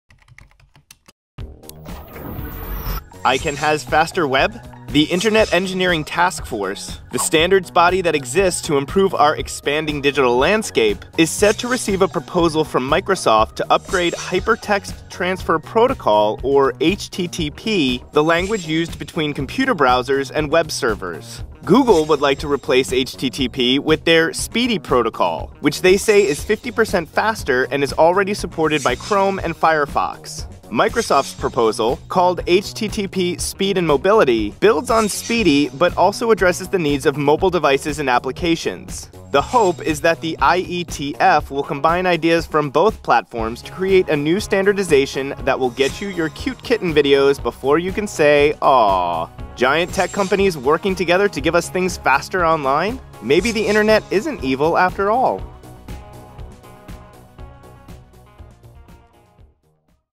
Microsoft's Faster Internet Plan: Browsing Made Better for Mobile?
I can haz faster web? The Internet Engineering Task Force, the standards body that exists to improve our expanding digital landscape, is set to receive a proposal from Microsoft to upgrade Hypertext Transfer Protocol, or HTTP, the language used between computer browsers and web servers.

Google would like to replace HTTP with their SPDY (PRONO—SPEEDY) protocol, which they say is 50% faster and is already supported by Chrome and Firefox. Microsoft's proposal, called HTTP Speed+Mobility, builds on SPDY but also addresses the needs of mobile devices and applications. The hope is that the IETF will combine ideas from both platforms to create a new standardization that will get you your cute kitten videos before you can say, "Awww!"

Giant tech companies working together to give us things faster online? Maybe the Internet isn't evil after all.